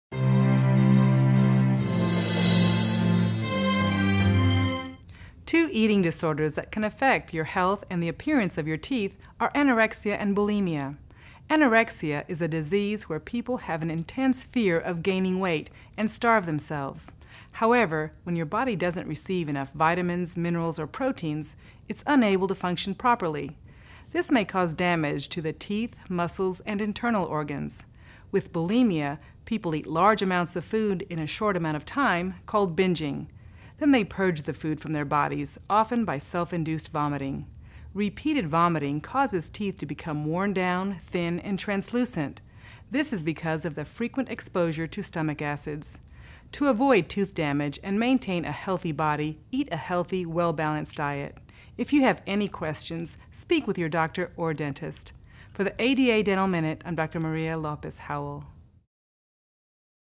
Anorexia & Bulimia
Two eating disorders that can affect your health and the appearance of your teeth are Anorexia and Bulimia. Anorexia is a disease where people have an intense fear of gaining weight and starve themselves. With Bulimia people eat large amounts of food in a short amount of time called binging and then they purge the food from their bodies often by self-induced vomiting. Learn more about how Anorexia and Bulimia can affect your teeth and oral health.